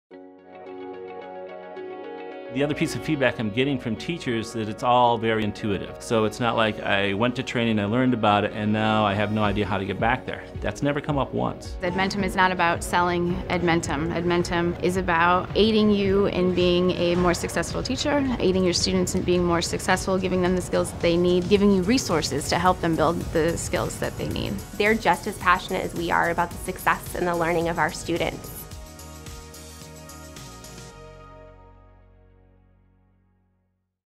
Student Success at Penn High School
Short clip on how Penn High School uses Edmentum for student success.